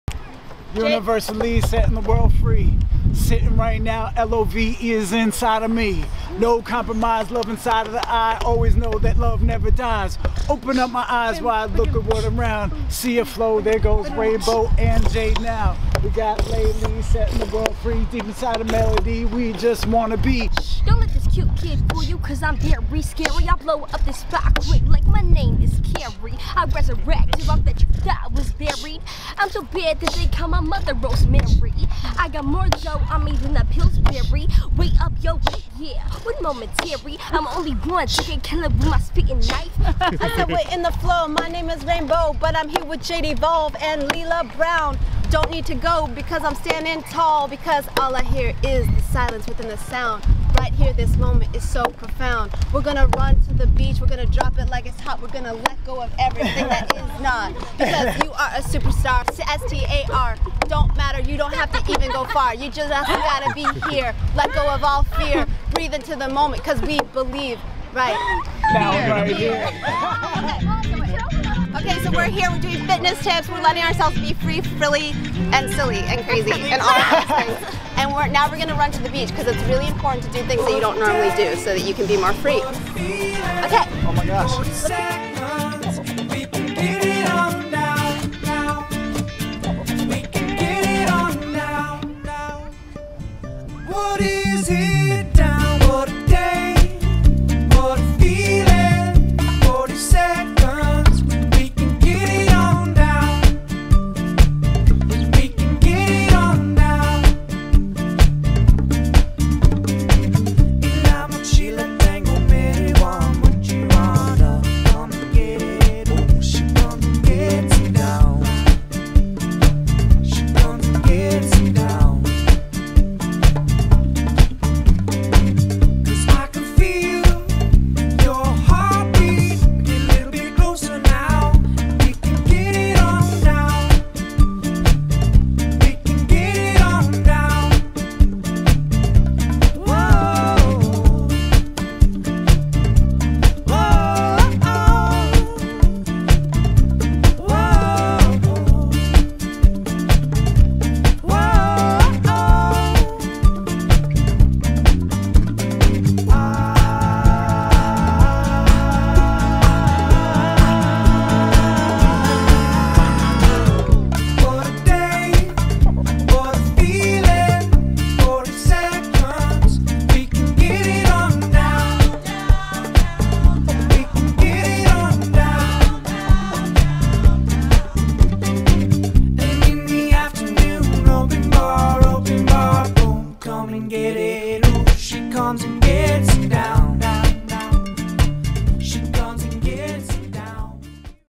Fitness fun at the beach & rapping
Join Johnny Sequoyah, Jade Mars, Lela Brown, Rainbeau Mars and Friends, Rickelle, Evolve and Lisa Callet Boise as they play in the sand and get a little silly. For more fitness and fun tips checkout Rainbeau's Book

Connect with Rainbeau for more Inspiration Daily!